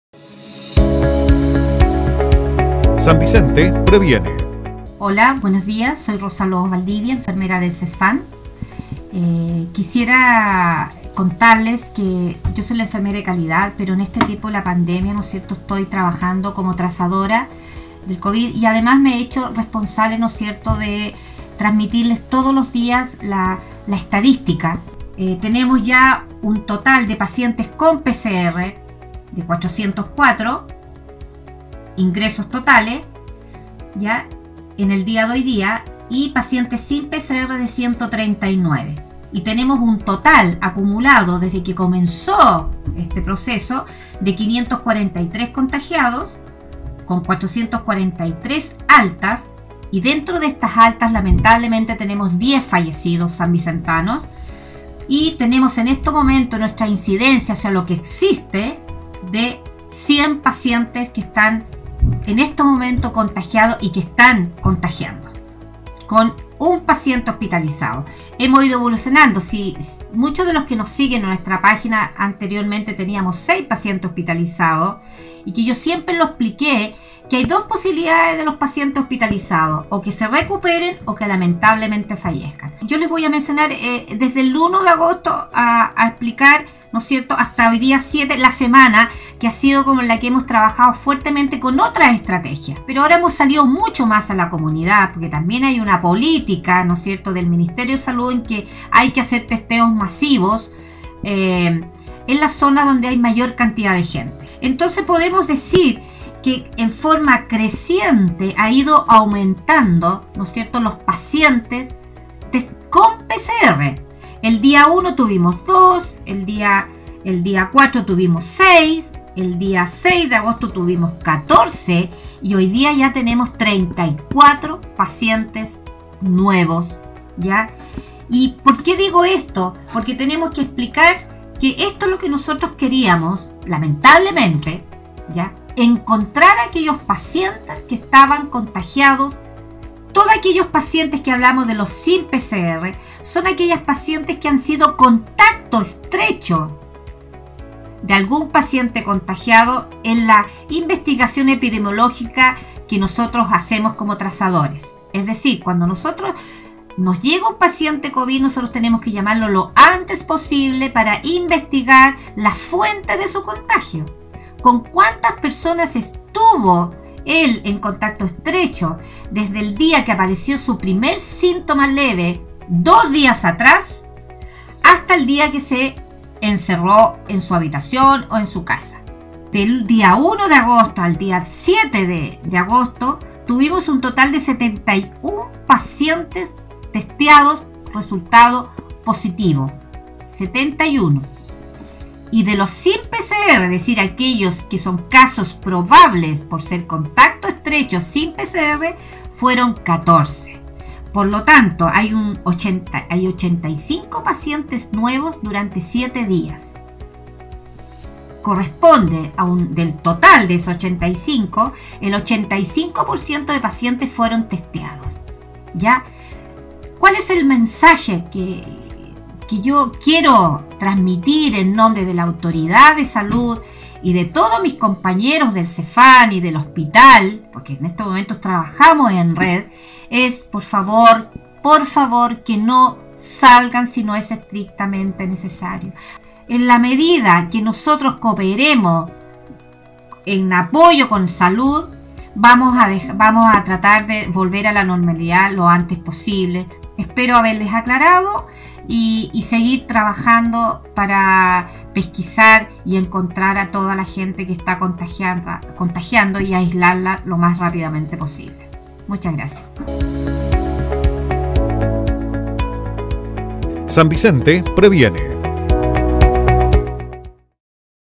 ENFERMERA DE TRAZABILIDAD EXPLICA NÚMERO DE CONTAGIOS Y LLAMA AL AUTOCUIDADO
La enfermera encargada de Calidad e integrante del equipo de trazabilidad del Cesfam, Rosa Lobos Valdivia, explica la metodología de seguimiento y trazabilidad de pacientes COVID-19 que se está realizando y que ha dado como resultado un alto y creciente número de personas contagiadas en la comuna, lo que preocupa tanto a las autoridades locales como a los vecinos.

La profesional refirió que en las últimas semanas se ha puesto en marcha una nueva estrategia, que es la de testeo masivo con PCR, a grupos y sectores que presentan mayor riesgo de adquirir la enfermedad y también a la población en general. Con esto, se pretende detectar a las personas asintomáticas y, de este modo, “cortar esta cadena” y disminuir los contagios.

Finalmente, destacó que tanto las autoridades como los profesionales de la salud necesitan de la colaboración de la comunidad para bajar la taza de contagios. “No salga si no es estrictamente necesario. Necesitamos su apoyo para volver a la normalidad y no tener que lamentar muertes y la pérdida de más trabajos”, advirtió.